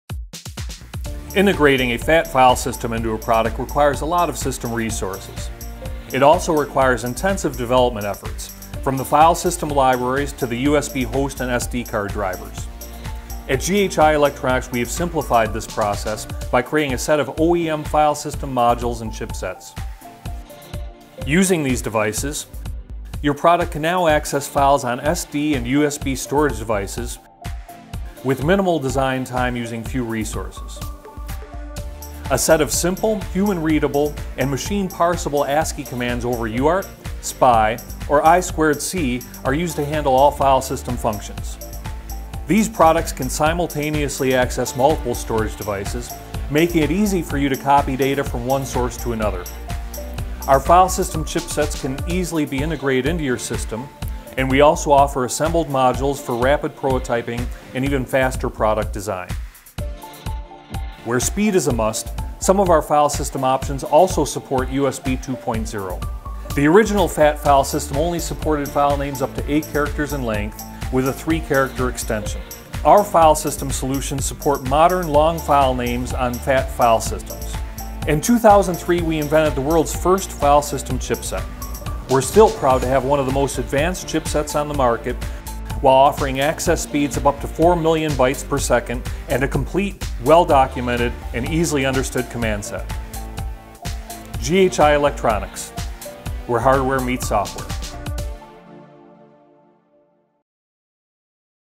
Integrating a FAT File System into your Design
GHI Electronics have simplified this process by creating a set of OEM Modules and chipsets.